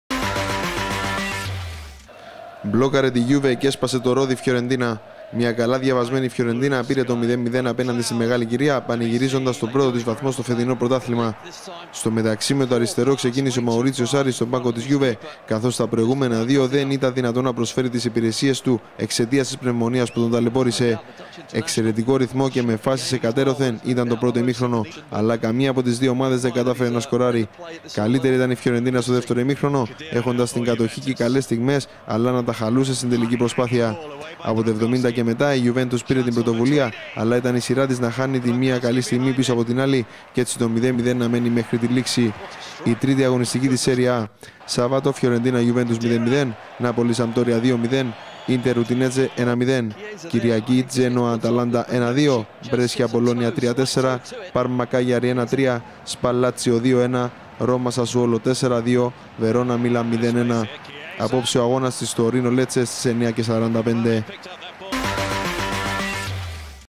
Έπεσε σε… μπλόκο στη Φλωρεντία η Γιουβέντους!
Η 3η αγωνιστική της Serie A
Σάββατο, 14/9
Φιορεντίνα-Γιουβέντους 0-0
Νάπολι-Σαμπντόρια 2-0
Ίντερ-Ουντινέζε 1-0
Κυριακή, 15/9
Τζένοα-Αταλάντα 1-2
Μπρέσια-Μπολόνια 3-4
Πάρμα-Κάλιαρι 1-3
Σπαλ-Λάτσιο 2-1
Ρόμα-Σασουόλο 4-2
Βερόνα-Μίλαν 0-1
Δευτέρα, 16/9
Τορίνο-Λέτσε (21:45)